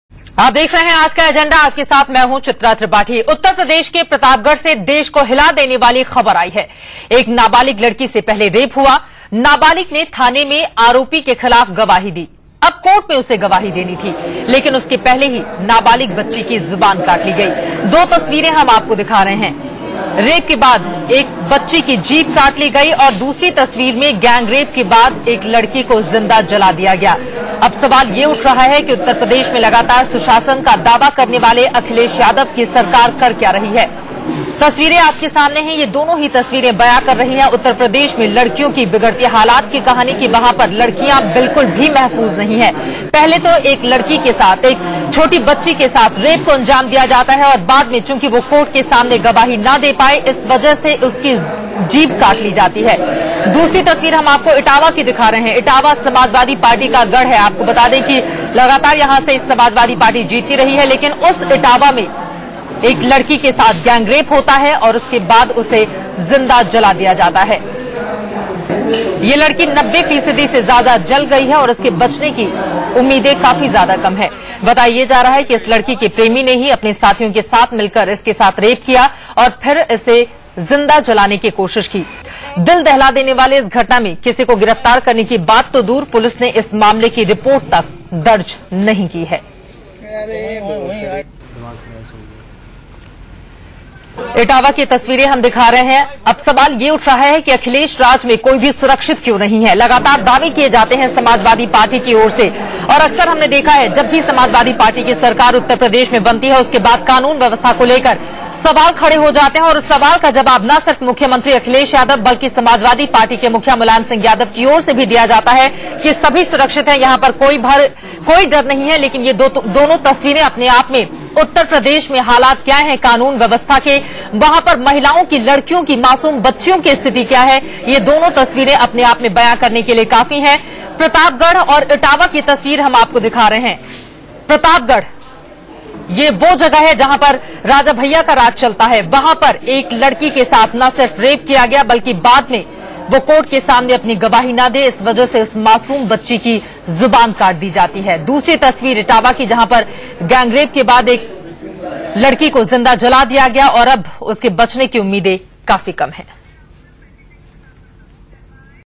India News: Plight of women in Uttar Pradesh
India News: The number of crimes against women is increasing in Uttar Pradesh. On one side, a minor was raped and her tongue was cut off in Pratapgarh, while on the other side a girl was gang raped in Ettawah which is the prime place of Samajwadi Party.